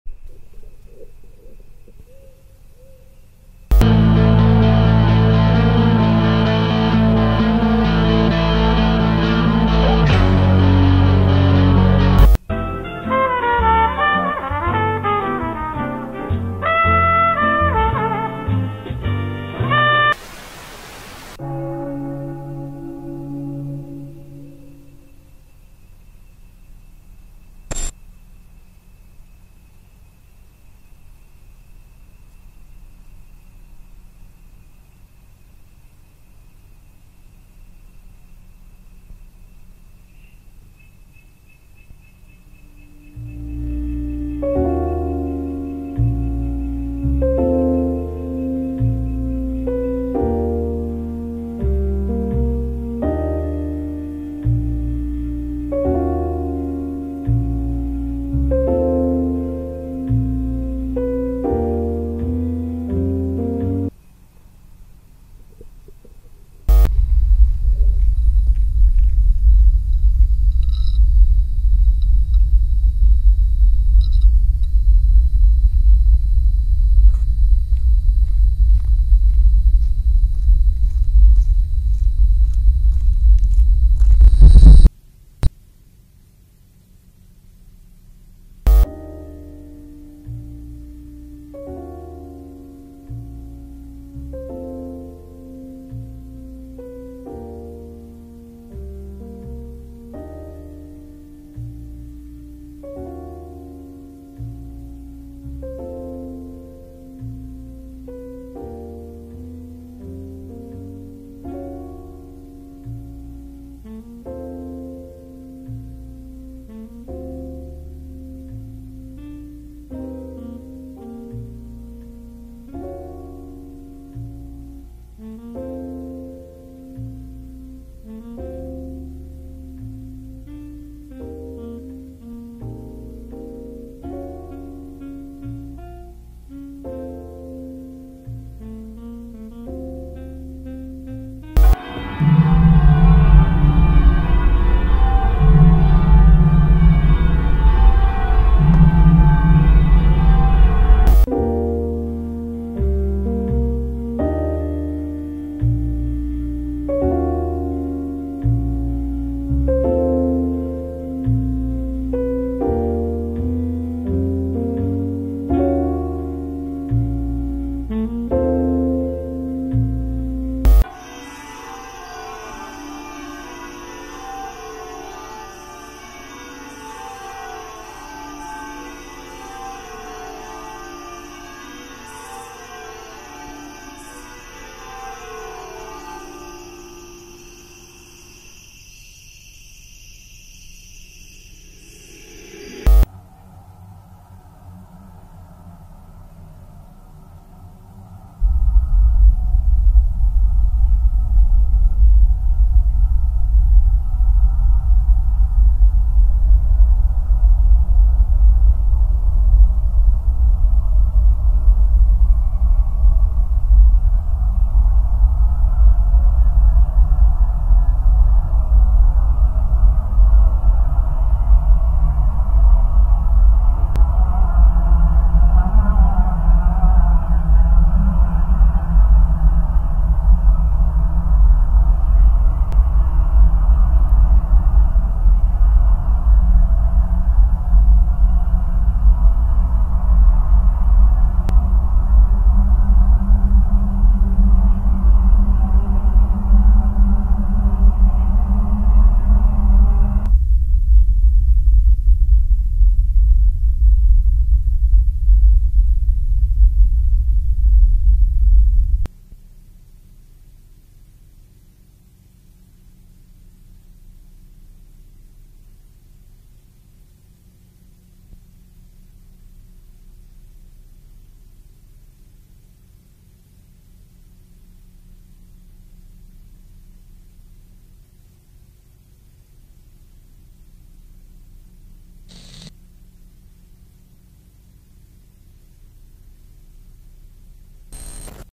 Surreal Broadcast - Groove Cross Annual Festival (1989)
Groove Cross is a very closed community, with its population decreasing each year.
Many disappearances occur in the region between the months of June and September.